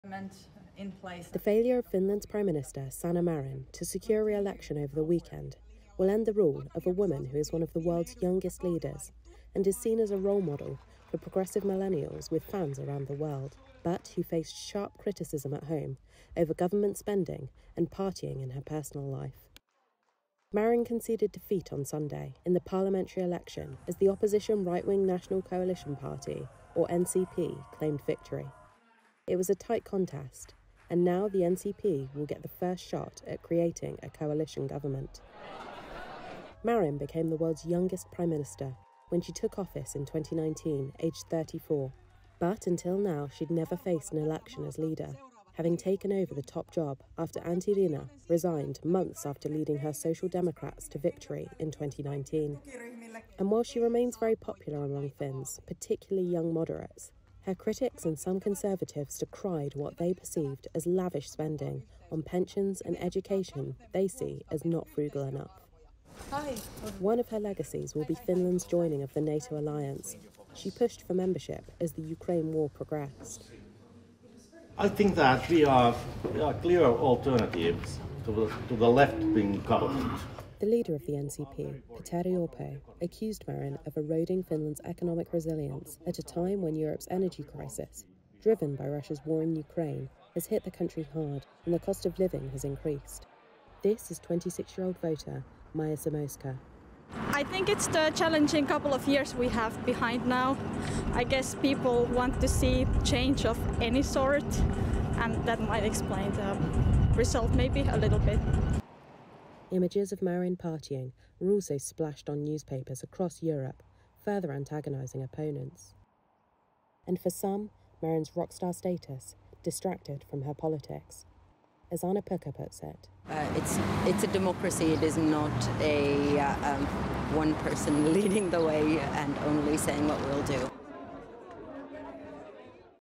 Why Finland voted out Sanna Marin, one of the world's youngest leaders
Finland's left-wing Prime Minister Sanna Marin conceded defeat on April 2 as the opposition right-wing National Coalition Party (NCP) claimed victory in a tightly fought contest. It ends the rule of a woman considered by fans around the globe as a millennial role model, but at home she has faced criticism for her partying and her government's public spending.